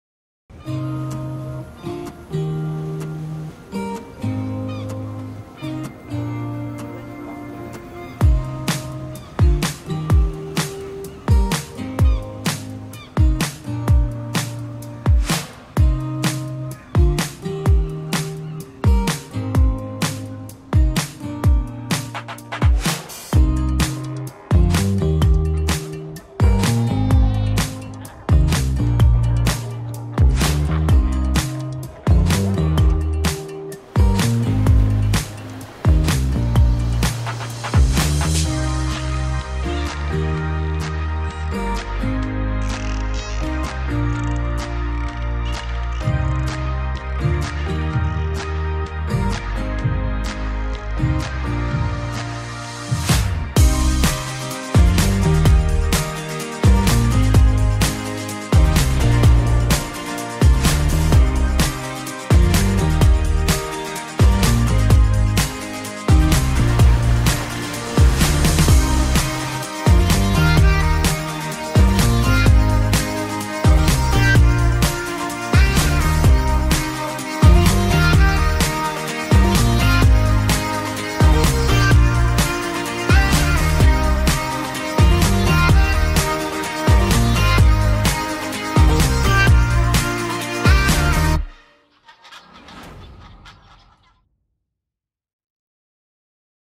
Catamaran Cruises with Namibian Marine Life | Laramon Tours
We filmed this exciting video production to help advertise an experience of a lifetime with Laramon Tours, taking tourists to experience a morning of marine cruising amongst seals and dolphins, in Walvis Bay.